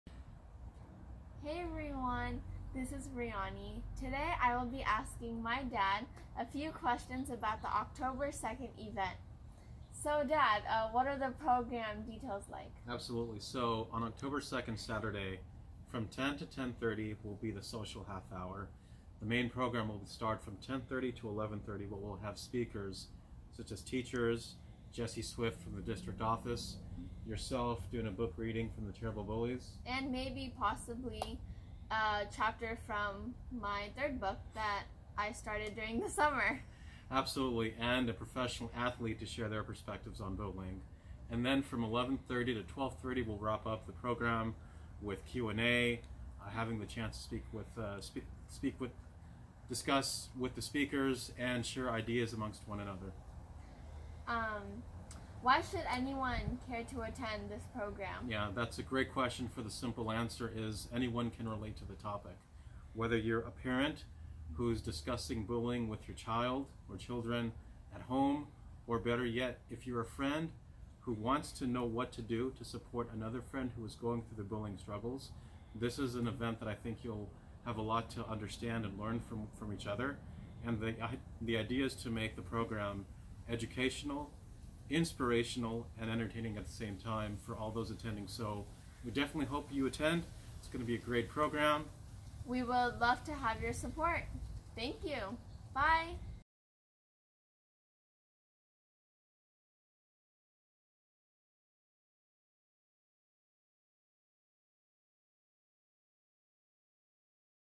Bullying Prevention Month Event at Morgan Hill Downtown Amphitheater
1 in 5 students report being bullied while in school. Many kids find it hard to express that they have been bullied but this event is for families to be with others that have like-minded experiences in discussing solutions and developing the courage and resolve to overcome their bullying struggles.

Join us at the Morgan Hill Downtown Amphitheater on October 2, 2021 from 10am - 12pm for discussions and strategy sessions with local teachers, psychologist, teen author Riyani Patel and a professional athlete.